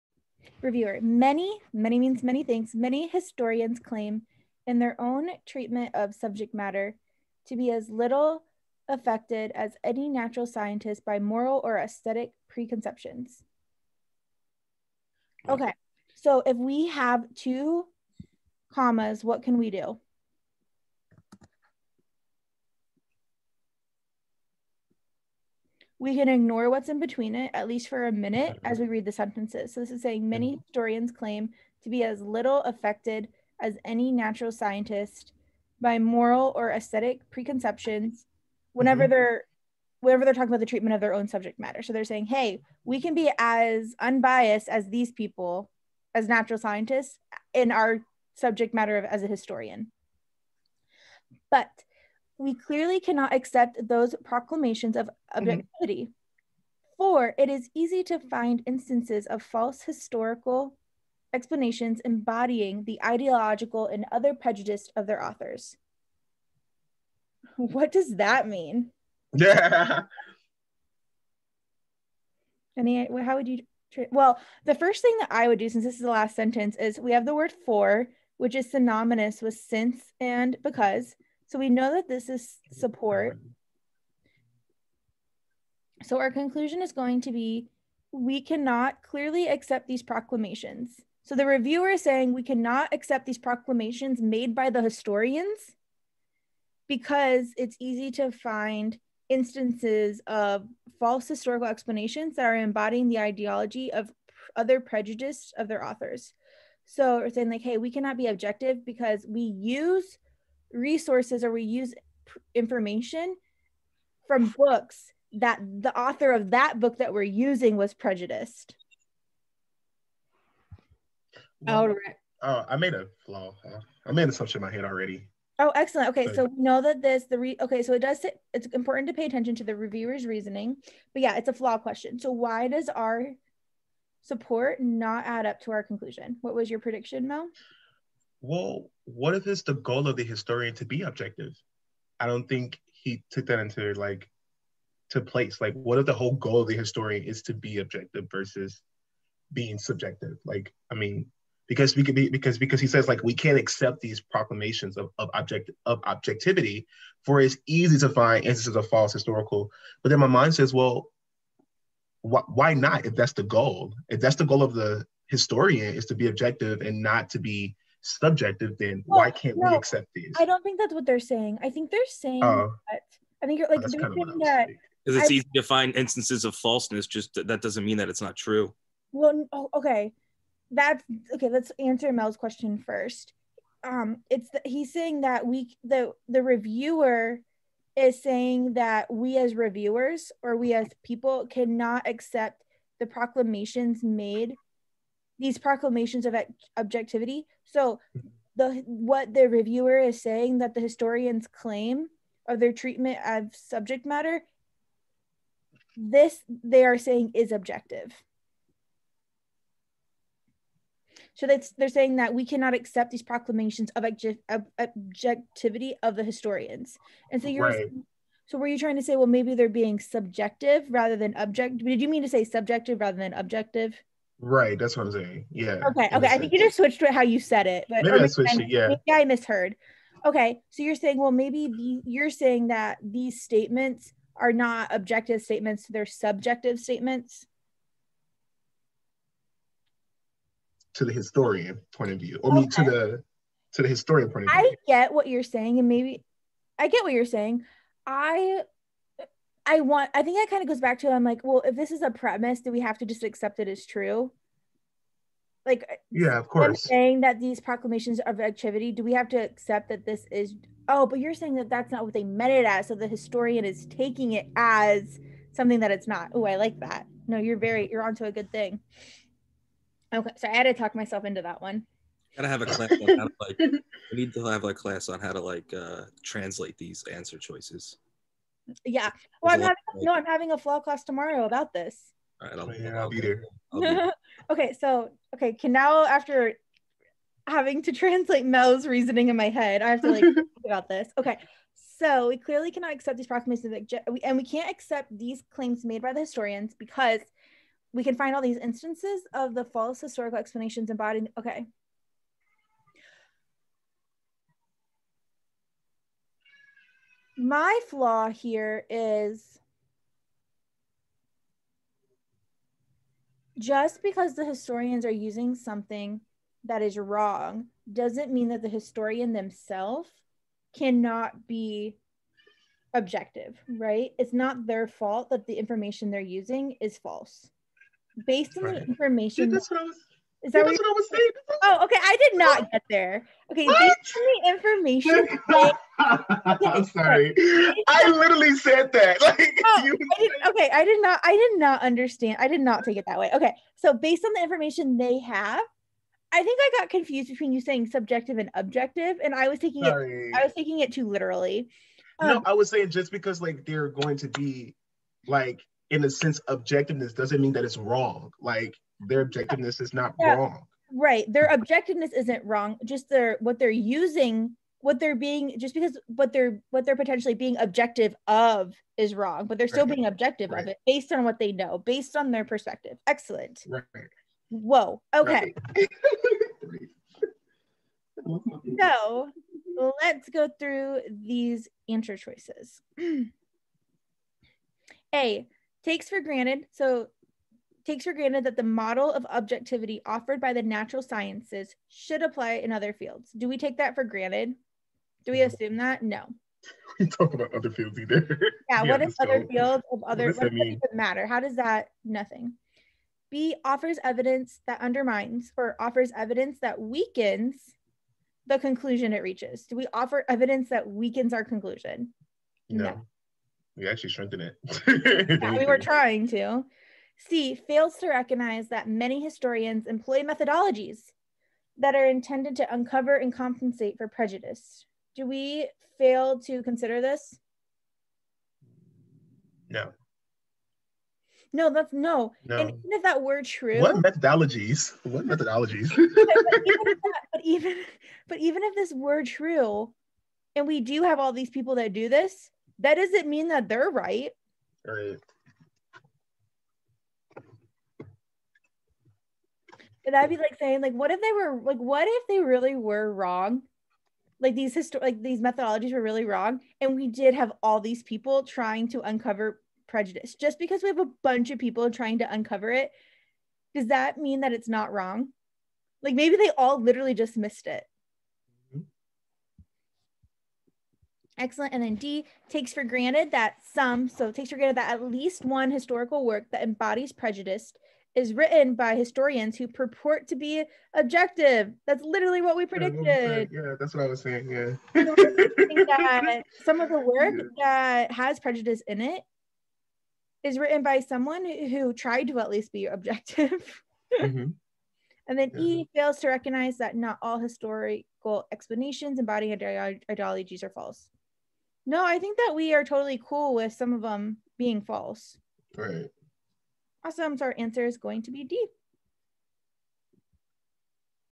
PT45 - LR1 - 20 (Flaw) (LRBT3-36)
PrepTest 45 -  December 2004 - LSAT Answers and Explanations

#20 Reviewer: Many historians claim, in their own

Favorite LSAT Resources:
The Loophole by Ellen Cassidy
LSAT Trainer by Mike Kim